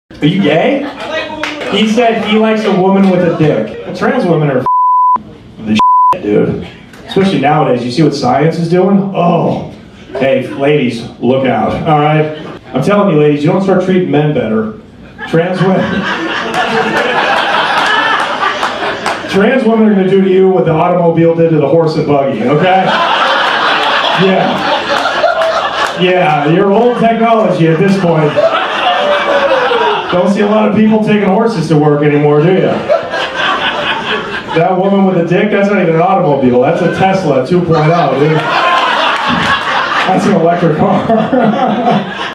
Will Transgender Women Replace Regular Women?? | Johnny Mitchell
Johnny Mitchell talks about why Trans women will replace real women in the future.